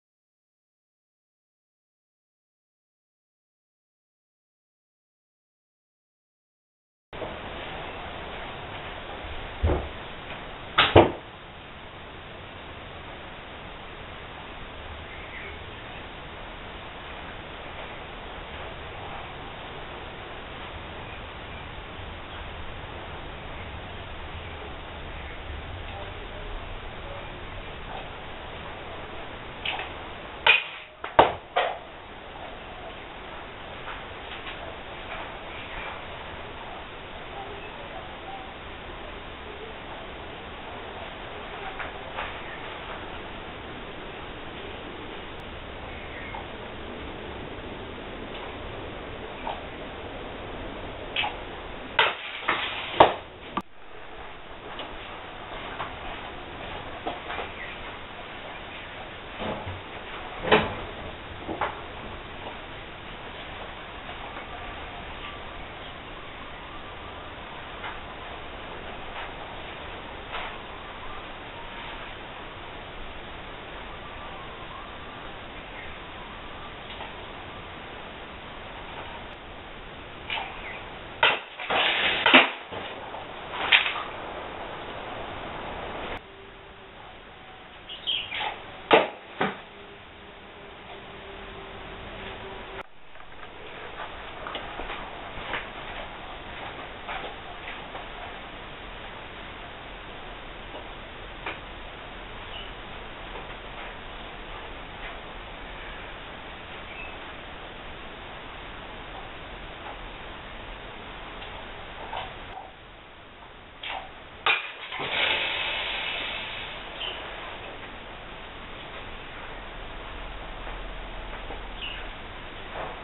6ShooterSoda
Umarex bb 6 shooter on soda cans.